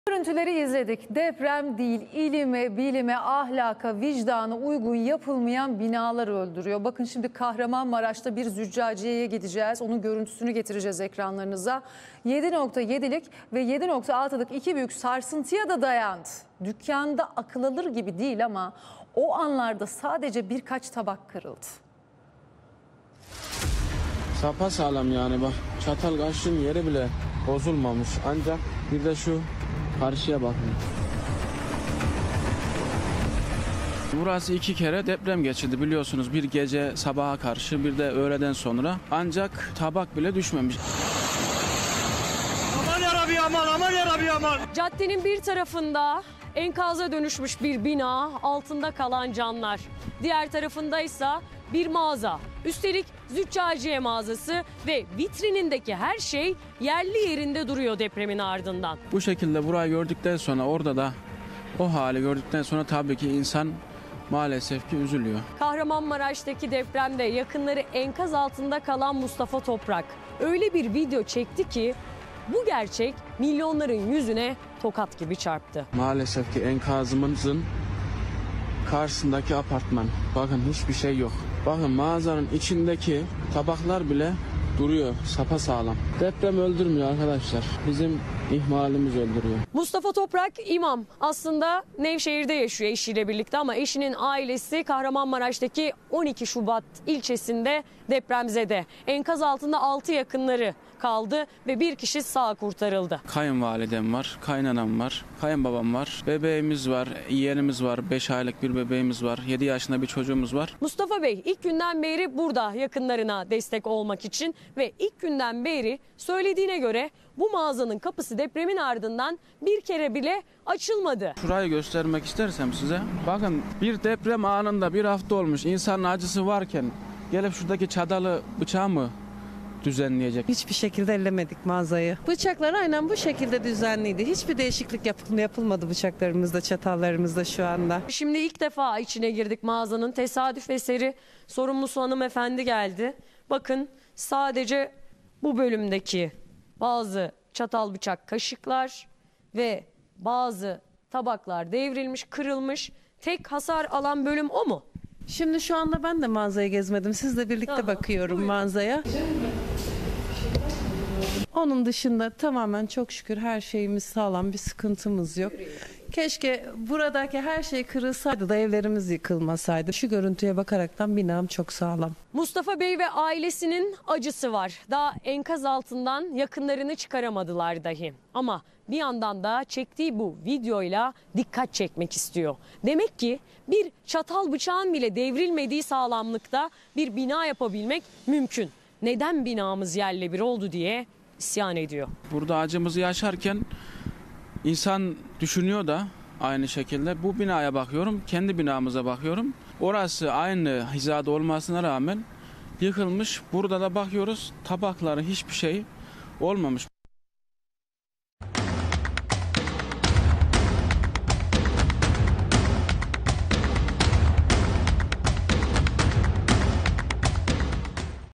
Züccaciye dükkanı sapasağlam kaldı
KAHRAMANMARAŞ'TA BİR ZÜCCACİYECİ , 7.7 VE 7.6'LİK İKİ BÜYÜK SARSINTIYA DA DAYANDI. DÜKKANDA AKILALIR GİBİ DEĞİL AMA O ANLARDA SADECE BİRKAÇ TABAK KIRILDI... BİR KEZ DAHA  "DEPREM DEĞİL BİNA ÖLDÜRÜYOR" DEDİRTTİ.